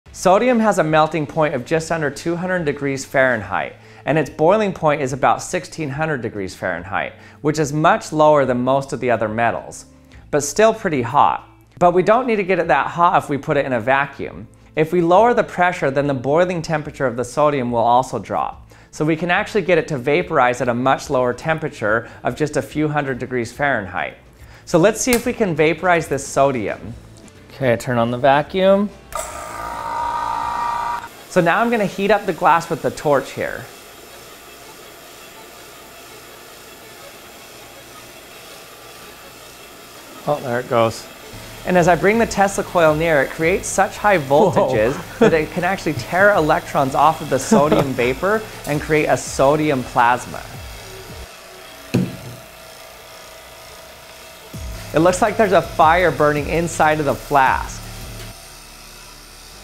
Boiling Metal To Make a Plasma
In this video I show you how I vaporize sodium in a vacuum to make a sodium vapor plasma